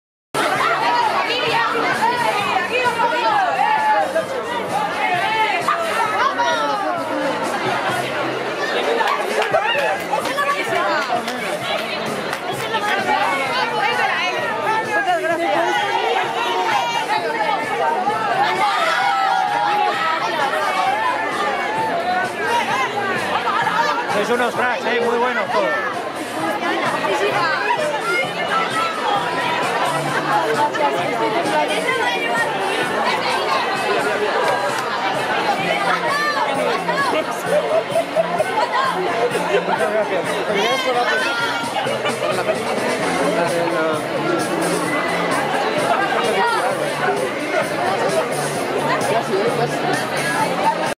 Zinemaldia 2022 - Alfombra roja: "Los reyes del mundo"
El equipo de la película "Los reyes del mundo", en la alfombra roja, en el festival internacional de cine de Donostia-San Sebastián, Zinemaldia 2022.